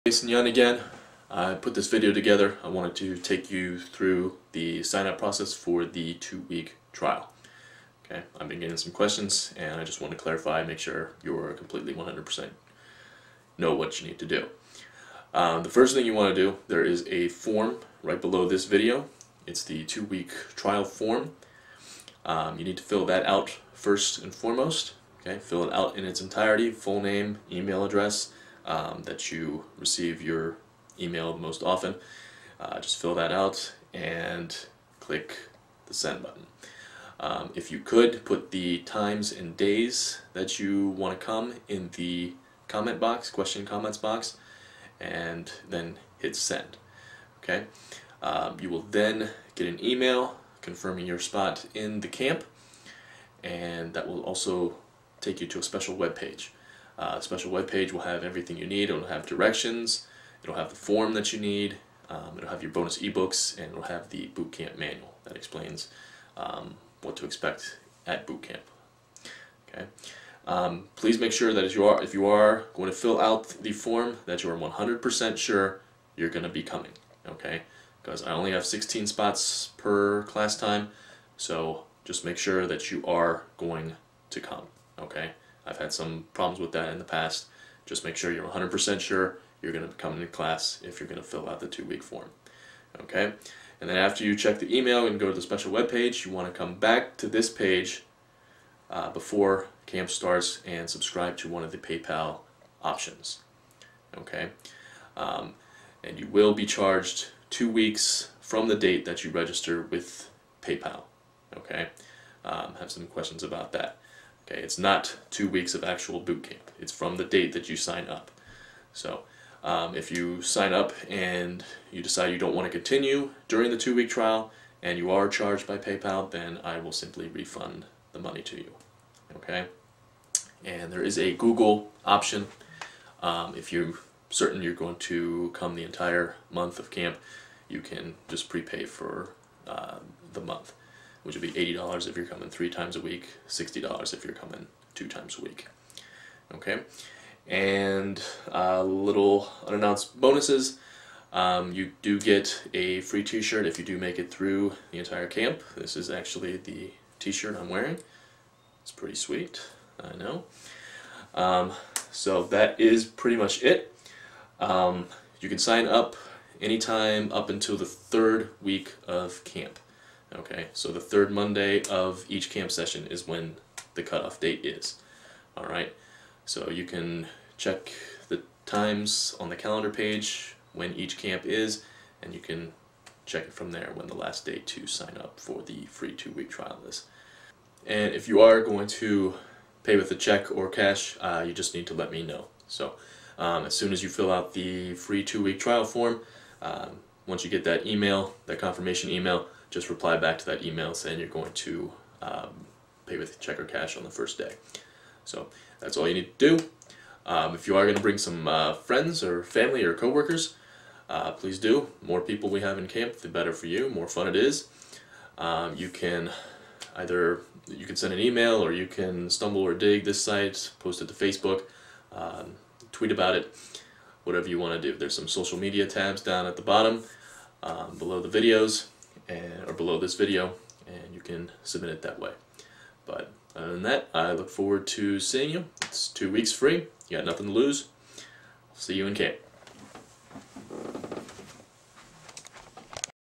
2-week free trial sign-up process for Columbus Boot Camp
Jason Yun explains how to sign up for his Columbus Fitness Bootcamp's 2-week trial.